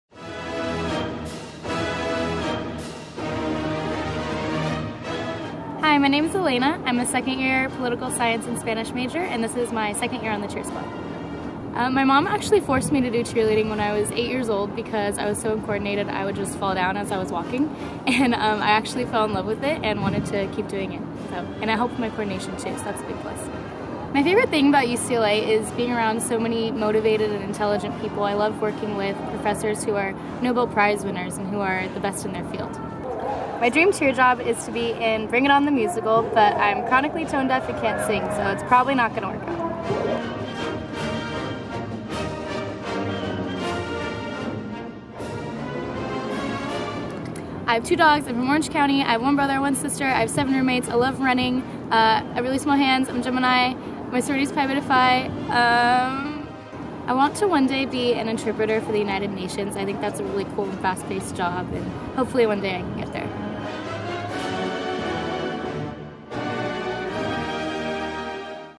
Meet Cheer Member Alayna!
Find out why Alayna loves UCLA!! She's a political science and spanish double major and we can't wait for you to meet her!